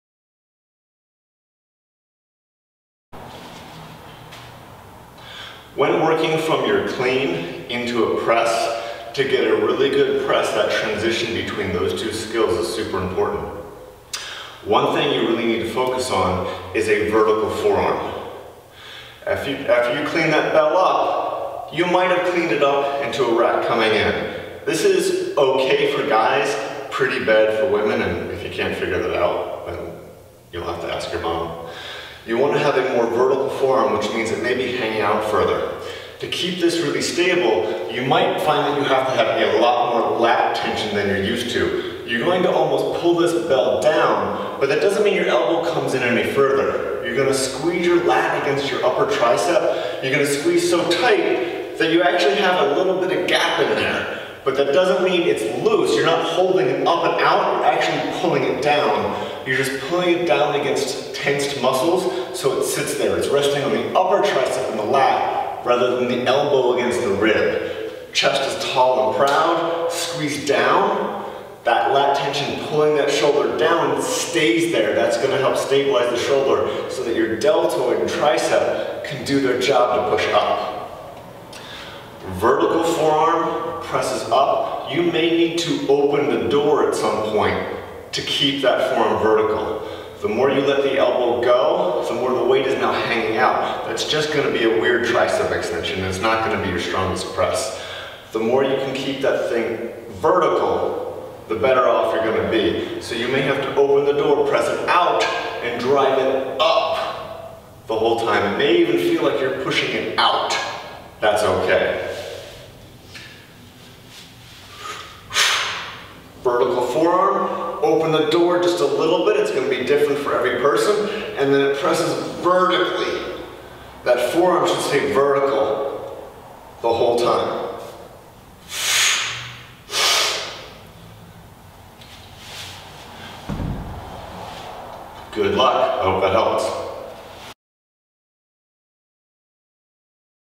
Transition from a clean to a kettlebell press.
The Kettlebell clean will be diffrent depending on the drill following it.  here is a video with some tips on what to do when transitioning from a clean to a press.  with some solid tips on how to do a good kettlebell press.